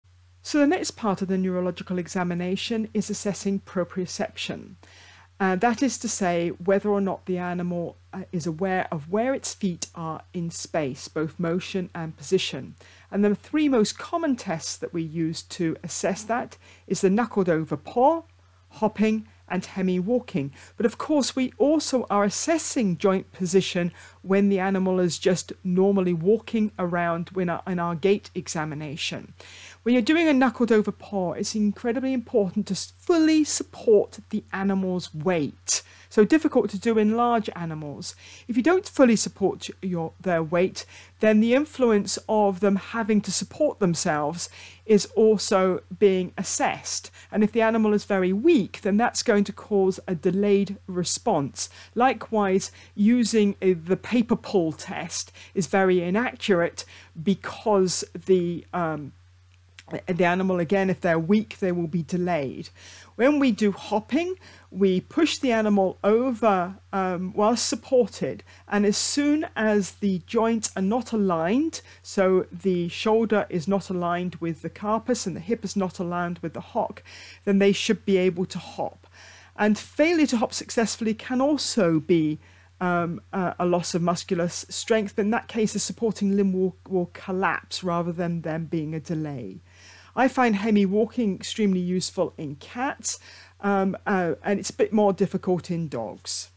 Neuro Exam 3 - proprioception
Proprioception and other postural reactions
Why
Assesses the integrity of the white matter tracts conveying sensory information to the cortex and also upper and lower motor neurons (strength). Proprioceptive and postural_deficits indicate neurological disease but are non-specific. Proprioceptive and postural deficits will be contralateral to the lesion above the level of the brain stem and ipsilateral below.
How
Assessed by observing the ability of the animal to replace a knuckled over paw or move an abducted or adducted paw back into position. In cats, ruminants and horses (unlike dogs) there has to be a severe damage to the proprioceptive pathways before there is failure to correct a knuckled over paw/hoof so this should not be the only evaluation of proprioception in these animals. The ability to correct paw_position is also dependant on the ability of the other limbs to support weight whilst the limb is lifted. The weak animal should be supported e.g. by an arm under abdomen otherwise the test can be over-interpreted. Pulling paper from under foot is a less unreliable method to assess proprioception.
Whilst supporting the contralateral limb (large animals) or body and three limbs (small animals) the animal is pushed gently sideways so that it hops on the remaining limb. As soon as the shoulder or hip moves from the perpendicular the animal should compensate for the imbalance by hopping the foot. Failure to hop successfully can be due to loss of muscular strength in which case the supporting limb will collapse, or a sensory lesion in which case there is a delay to hop. Normal proprioception but reduced strength, i.e. failure to hop but corrects knuckled paw when fully supported, is suggestive of neuromuscular disease.
The animal is lifted, supported and advanced towards a table with and without covering the eyes. When the normal animal see or feels the table they should advance both feet placing them on the table. This test can be useful to detect subtle lesions
The appropriate half of the animal is supported and function of the other limbs is assessed. This is also useful to detect subtle lesions and assess symmetry.